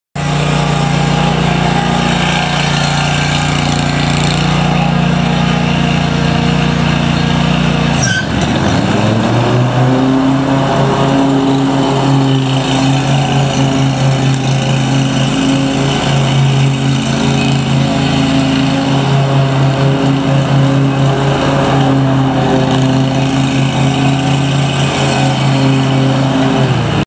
Kubota Z411 ZTR lawn mower for sale at auction | bidding closes January 12, 2022
Kubota Z411  ZTR lawn mower
 Hours: 166 on meter
 Cutting width: 48"
 Serial: KBGHCA0KKGL20919
Engine
 Kawasaki FS651V
 Cylinders: 2
 Fuel type: Gas
Transmission
 Hydrostatic
Tires
 Front: 13x5.00-6
 Rear: 24x9.50-14
Notes
 Deck bracket bent